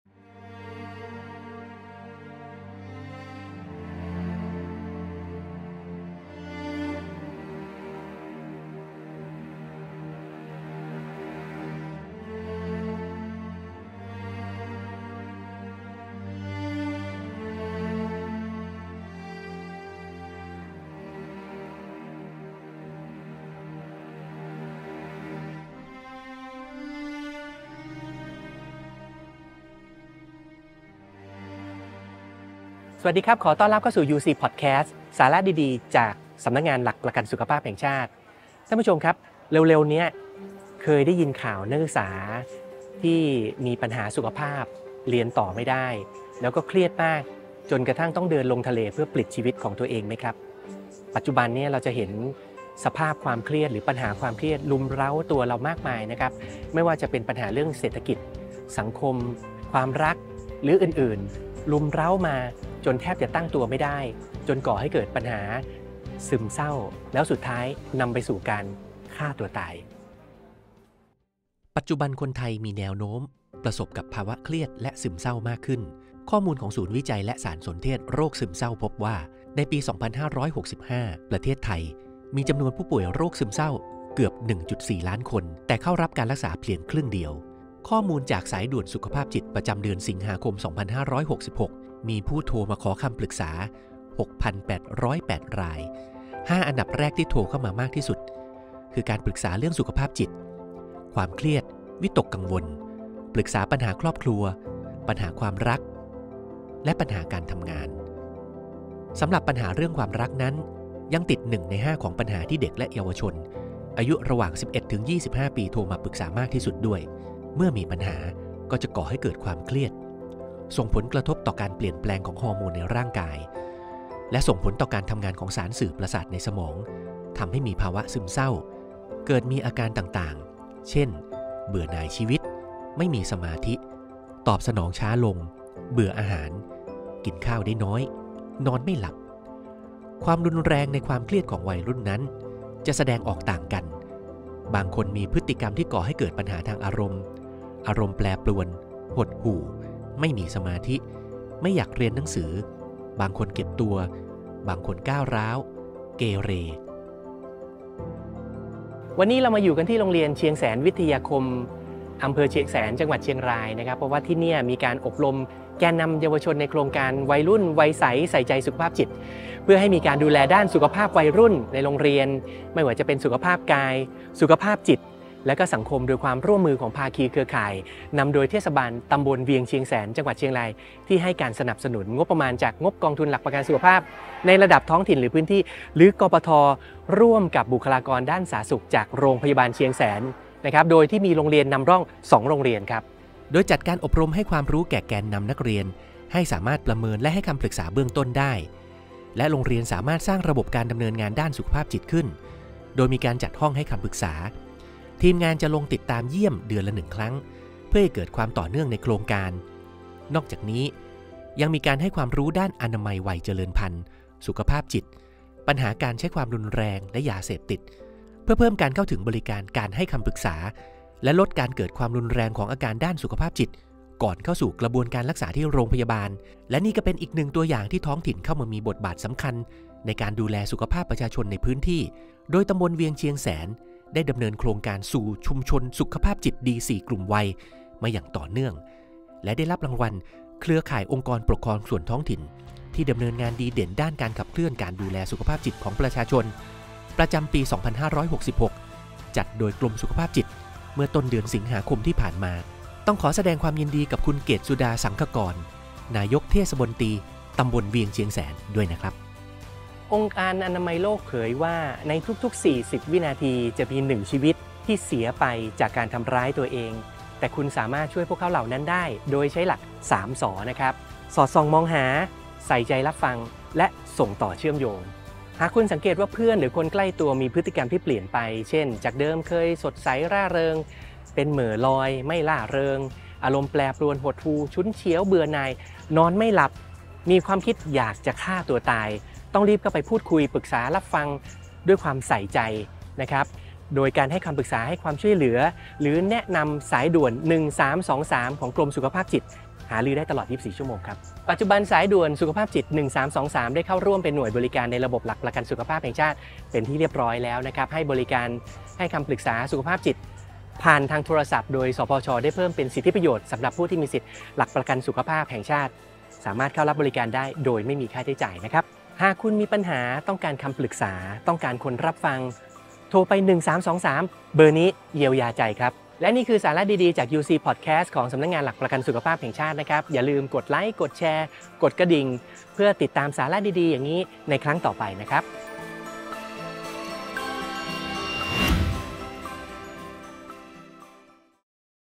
ตอนที่ 48 1323... เบอร์นี้เยียวยาใจ
ปัจจุบัน สายด่วนสุขภาพจิต 1323 ได้เข้าร่วมเป็นหน่วยบริการของสำนักงานหลักประกันสุขภาพแห่งชาติ  หรือ สปสช. เพื่อให้บริการปรึกษาปัญหาสุขภาพจิตผ่านทางโทรศัพท์  โดย สปสช. ได้เพิ่มเป็นสิทธิประโยชน์ด้านการให้คำปรึกษาปัญหาสุขภาพจิต ฟรี  สำหรับคนไทยทุกสิทธิการรักษา เป็นการเพิ่มการเข้าถึงบริการ โดยไม่มีค่าใช้จ่าย
หากคุณมีปัญหา ต้องการคำปรึกษา ต้องการคนรับฟัง โทรไป 1323 เบอร์นี้เยียวยาใจ ครับ